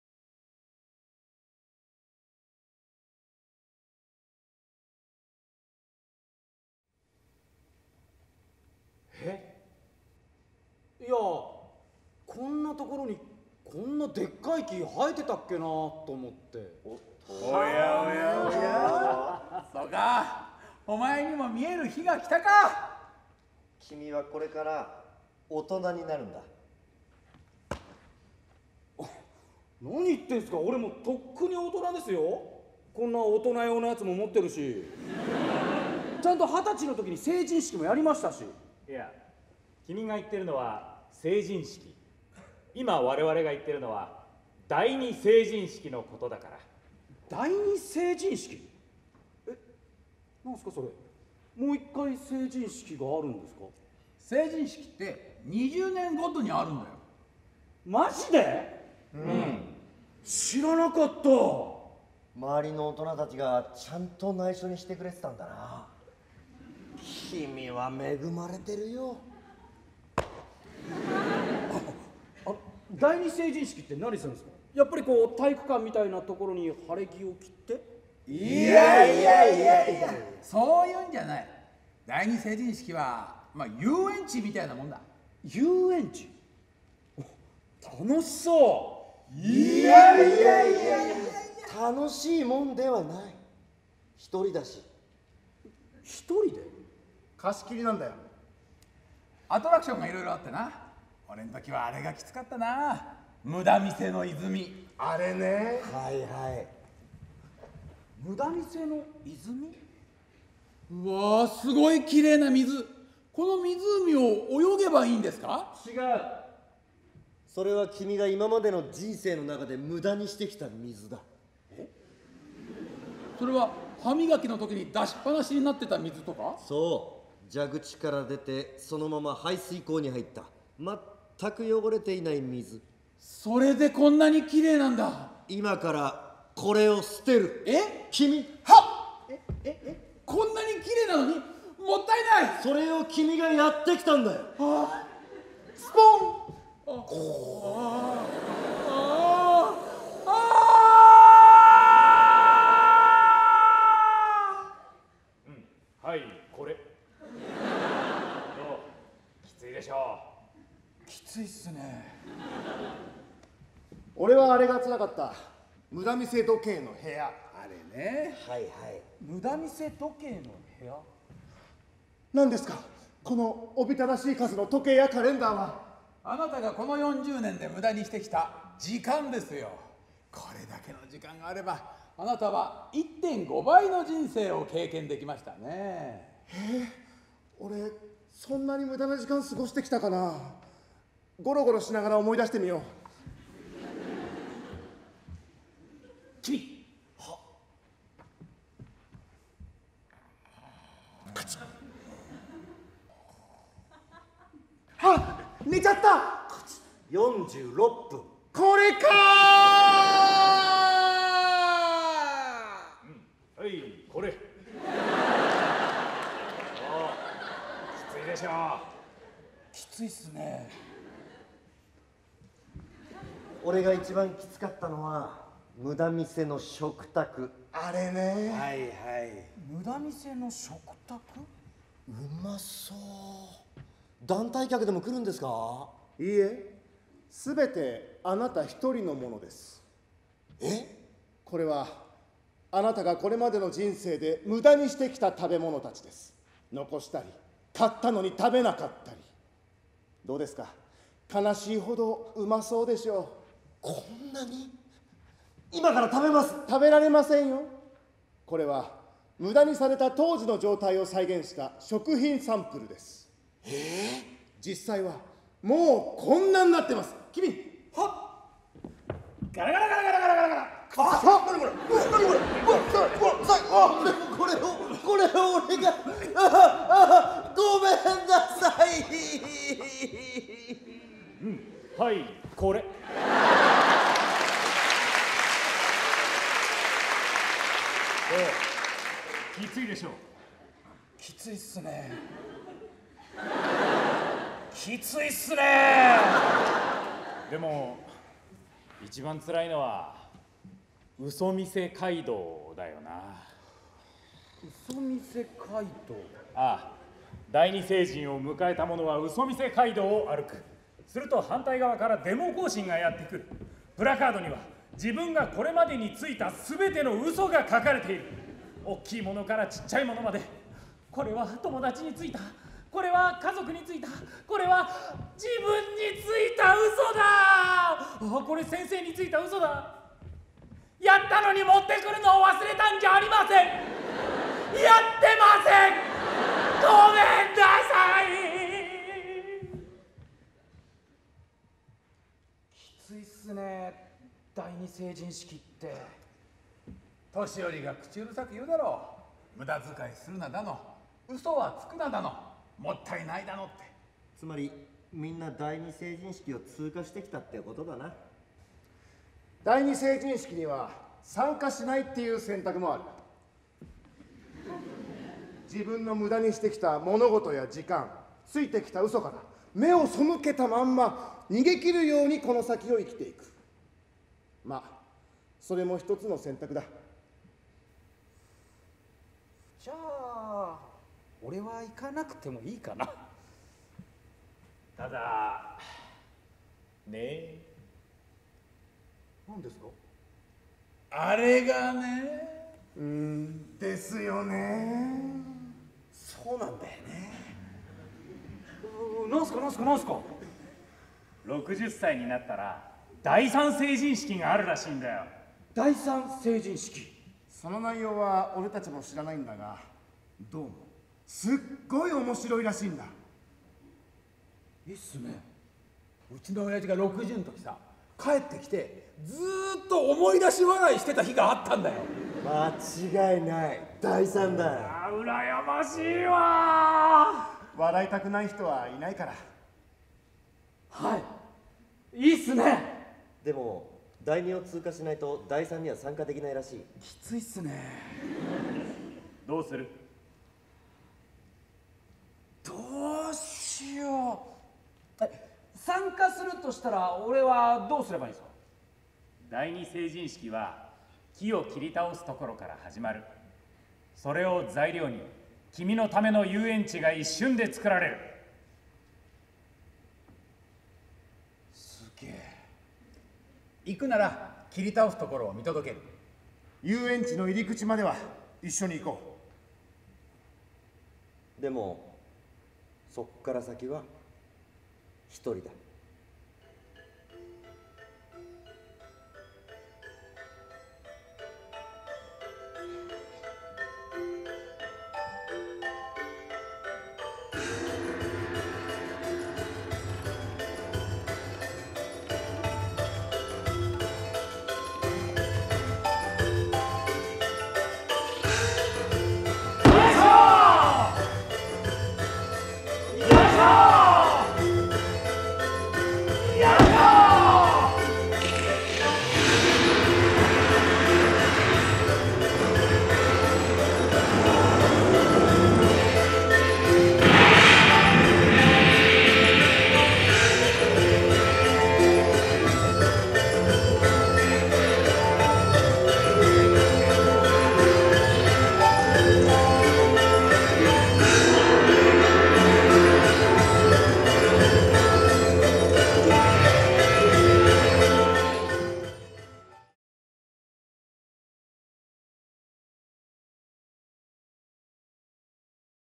カジャラ＃１『大人たるもの』より「第二成人式」
コント集団カジャラ
＃１『大人たるもの』より「第二成人式」
作・演出：小林賢太郎
出演：片桐仁／竹井亮介／安井順平／辻本耕志／小林賢太郎